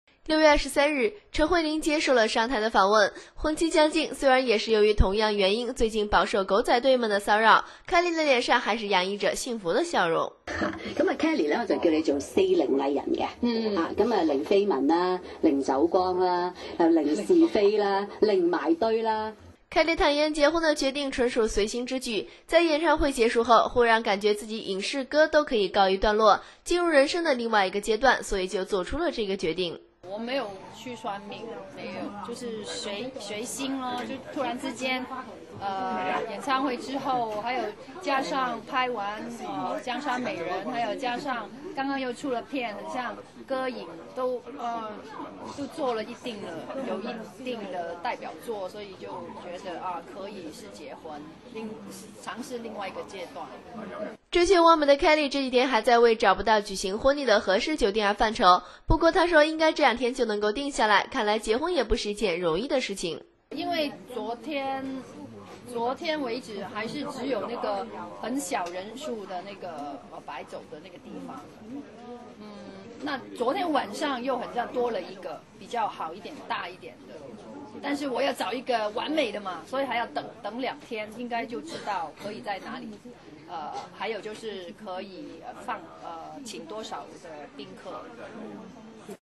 陳慧琳接受商台訪問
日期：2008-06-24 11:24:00
新浪娛樂訊  陳慧琳接受了商台訪問。婚期將近，雖然也是由於同樣原因最近飽受狗仔隊們的騷擾，KELLY的臉上還是洋溢著幸福的笑容。

KELLY坦言，結婚的決定純屬隨心之舉，在演唱會結束後，忽然感覺自己的影視歌都可以告一段落，進入人生的另外一個階段了，所以就做出了這個決定。

追求完美的KELLY這幾天還在爲找不到舉行婚禮的合適酒店而犯愁，不過她說，應該這兩天就能夠定下來了，看來結婚也不是一件容易的事情。 TUNGSTAR/文并圖